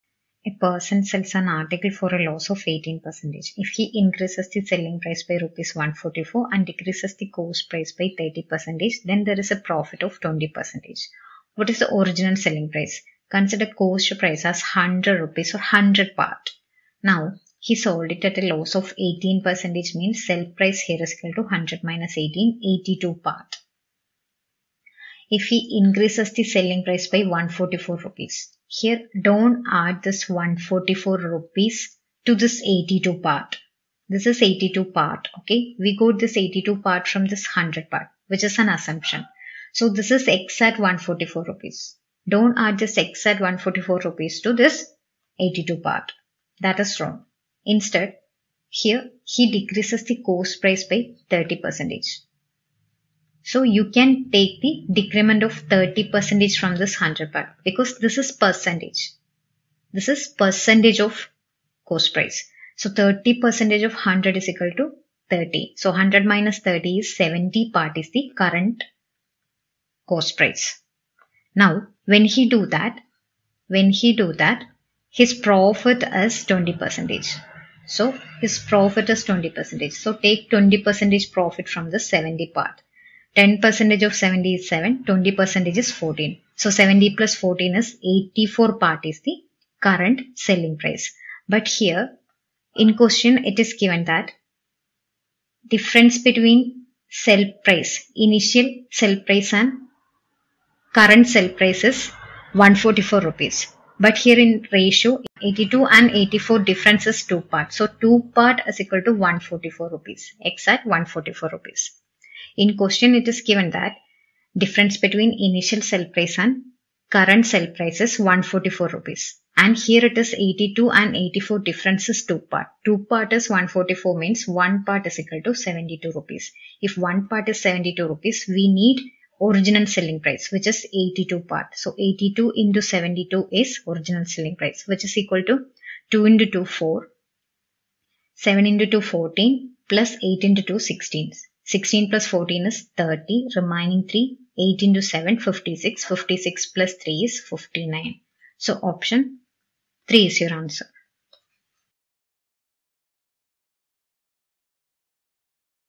A person sells an article for a loss of 18 percent. If he increases the selling price by Rs. 144 and
00:00 A person sells an article for a loss of 18 percent. If he increases the selling price by Rs. 144 and decreases the cost price by 30 percent, then there is profit of 20 percent. What is the original selling price?
1. Rs. 5068
2. Rs. 6036
3. Rs. 5904
4. Rs. 6124